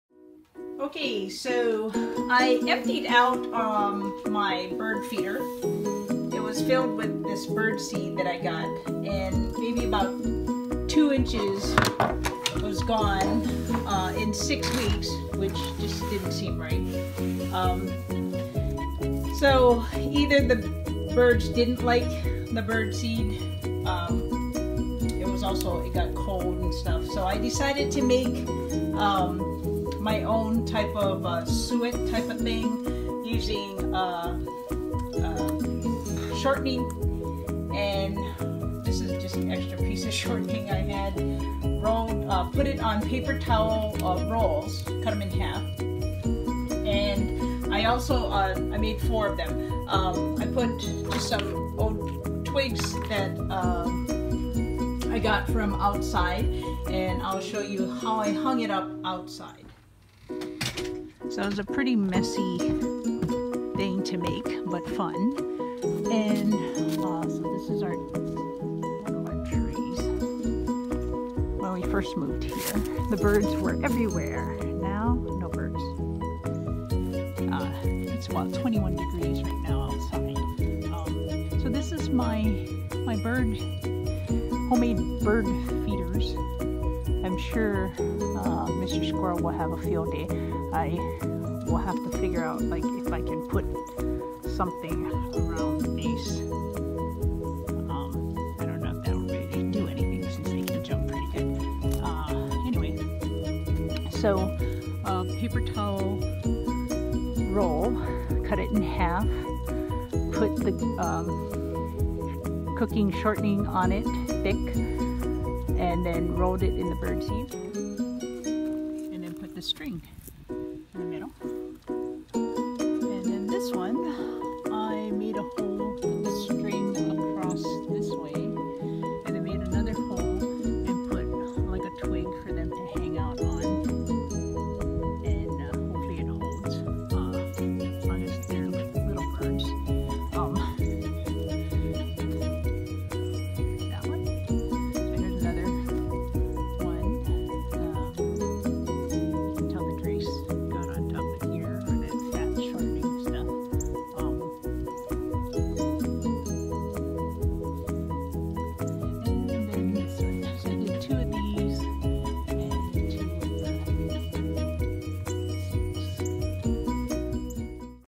DIY Suet Bird Feeders with Paper Rolls and Crisco Vegetable Shortening
How to make your own suet bird feeders using paper towel rolls, vegetable shortening, and most any birdseed, string & twigs. Look at 1:50 mark as I show the completed feeder and describe how I made it.